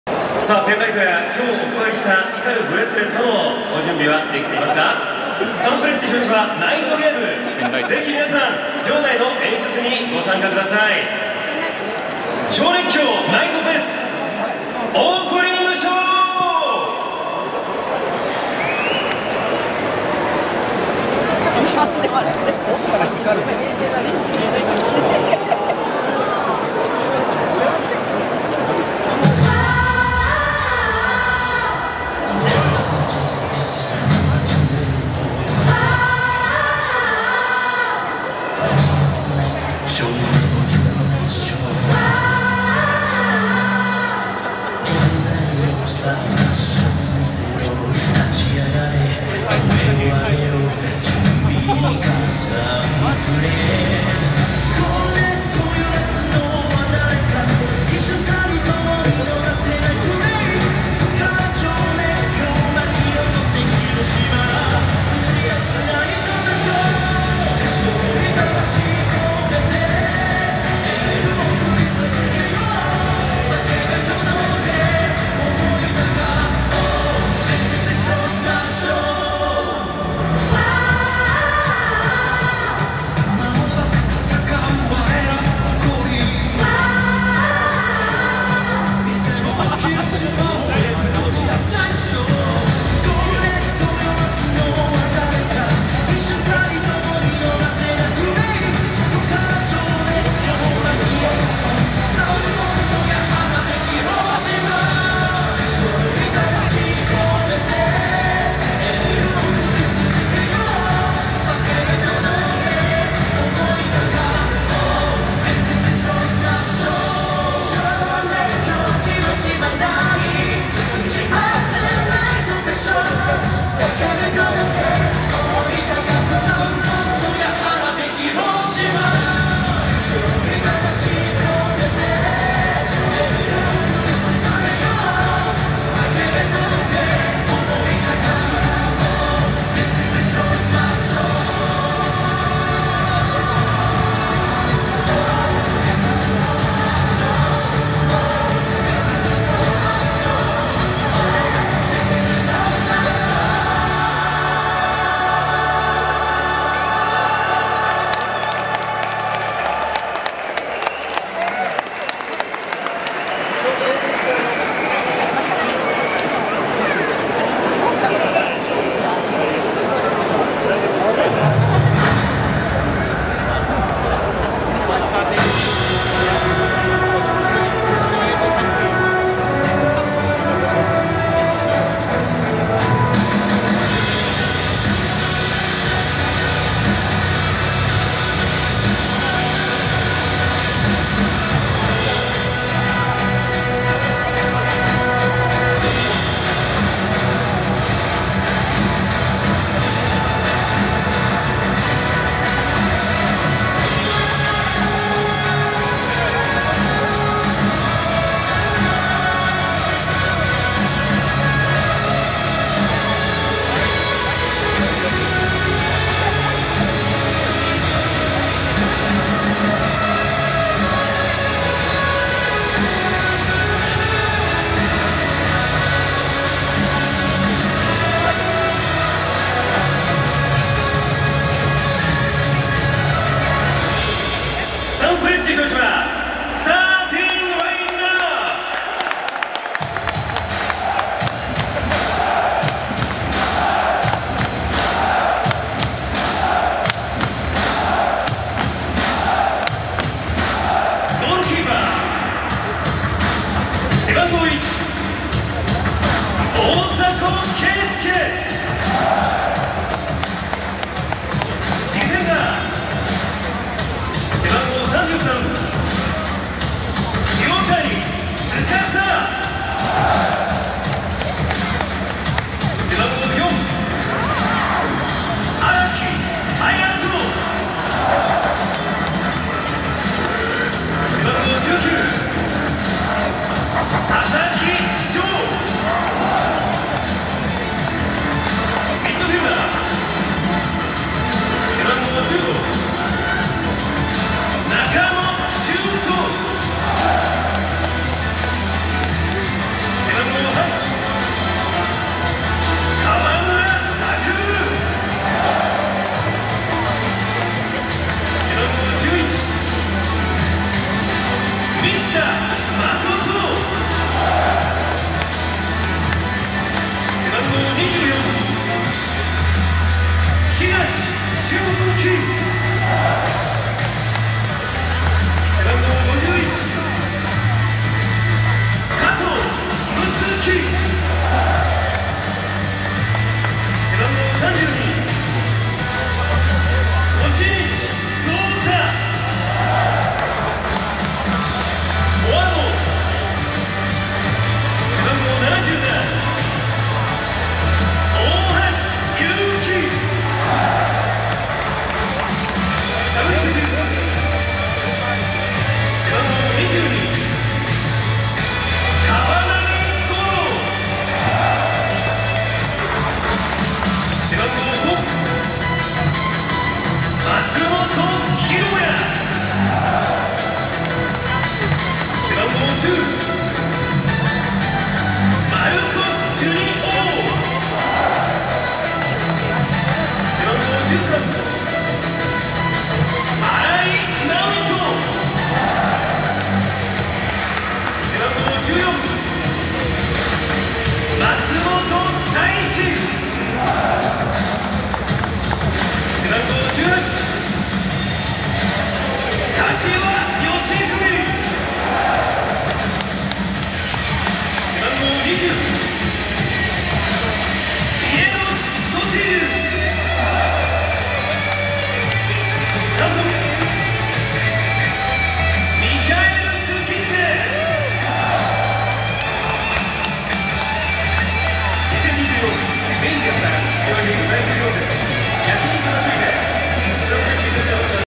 超熱狂NIGHT FESからの選手紹介
20240515にエディオンピースウイング広島で行われた初のナイトゲーム。超熱狂NIGHT FESとして盛り上がった選手紹介の様子